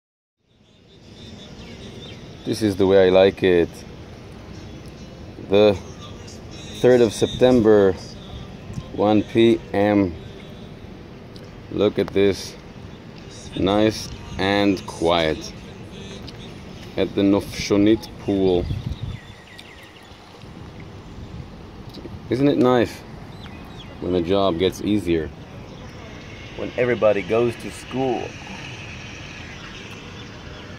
Life-guarding at Nofshonit Club pool in Holon
When the school-year begins the load of kids and parents subsides..